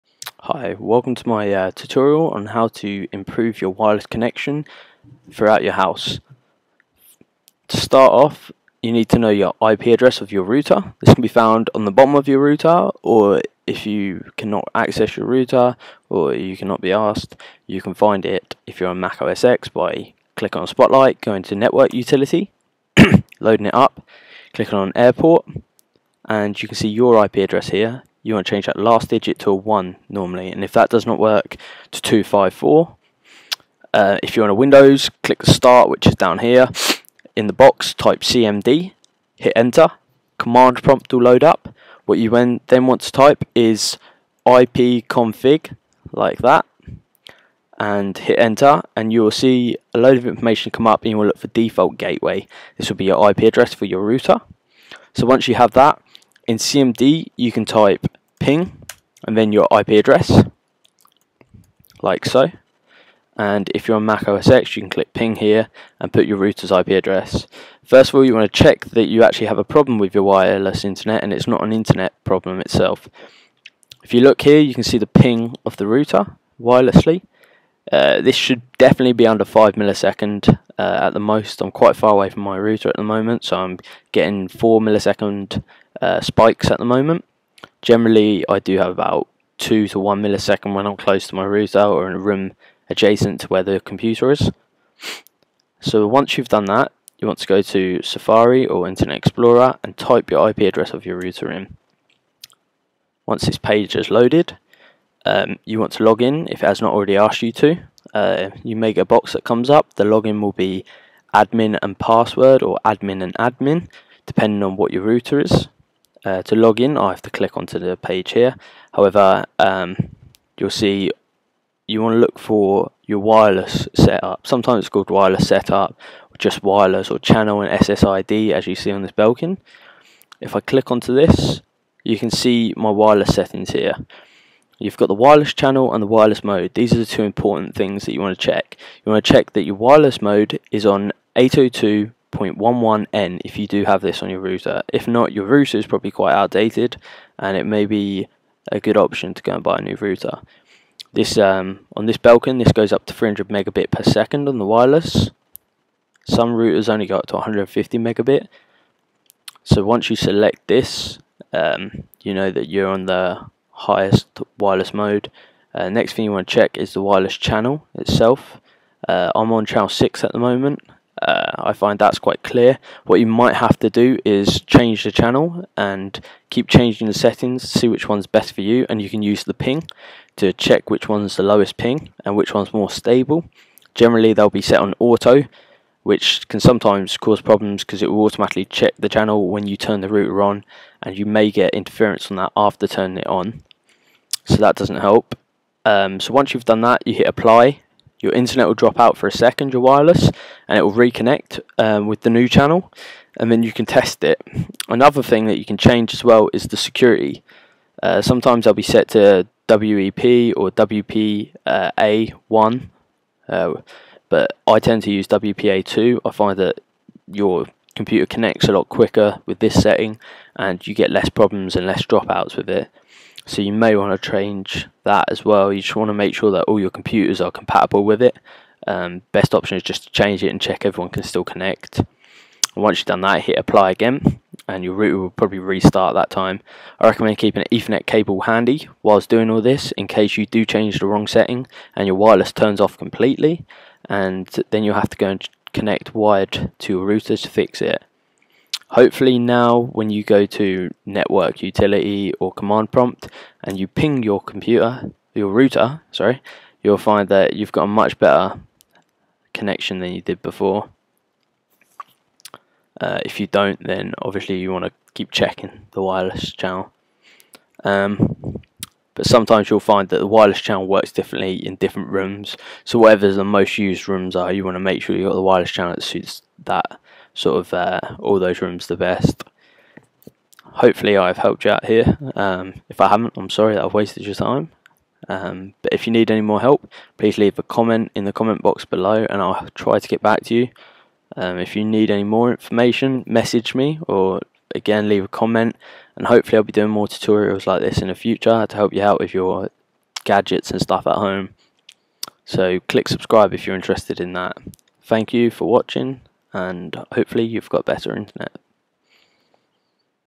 How To Improve Your Wireless Network Signal For FREE (Improve Wifi)
In this video I show you how you can easily improve your wireless connection at home by changing the settings that broadband companies don't tell you to change.

Don't Spend Money on New Routers and New Hardware when it may not be the problem!
Use this 1 free tip!

Mac OSX: Use Network Utility to find IP and to ping.
Windows: Press Start and Type CMD and hit enter to open command prompt to find the IP of your router (ipconfig) and to ping (ping 192.168.x.x)

The reason this helps your wireless network is that some wireless channels may have interference in your house or workplace and make your connection 'lag' and 'spike'.
By changing the wireless channel you can stop this happening and make your connection more stable and useable.

Thanks